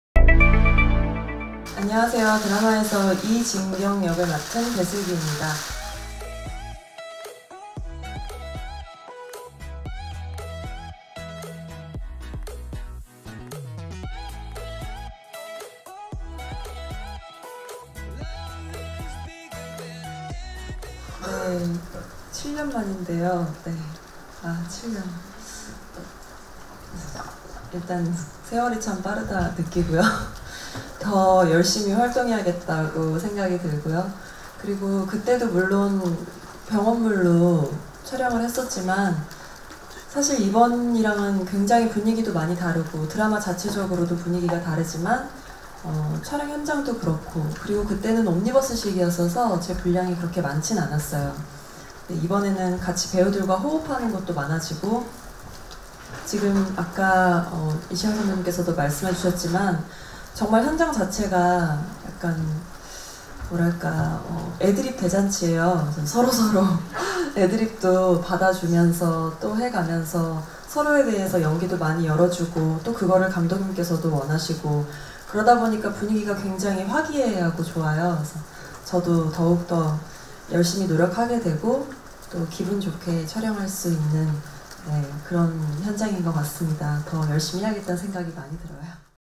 enewstv ′사생결단 로맨스′ 배슬기, 7년만에 병원물로 컴백 '세월 참 빠르다' 180719 EP.145
19일 오후 서울 마포구 상암 MBC에서 ‘사생결단 로맨스’ 제작발표회가 진행됐다. 이 자리에는 연출을 맡은 이창한 PD를 비롯해 지현우, 이시영, 김진엽, 윤주희, 선우선, 배슬기가 참석했다.

- 영상연출: 김지훈